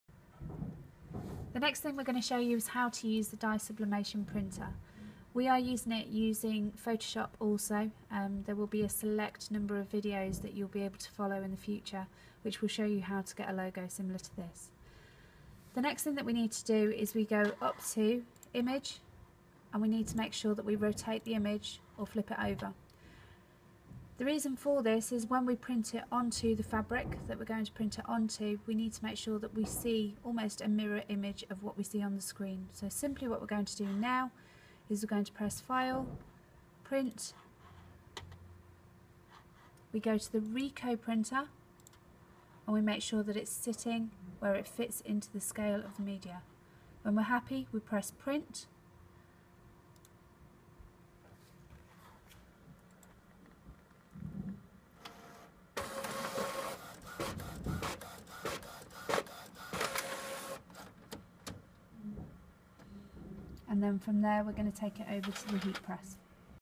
Dye Sublimation
How to create a logo and print onto fabric.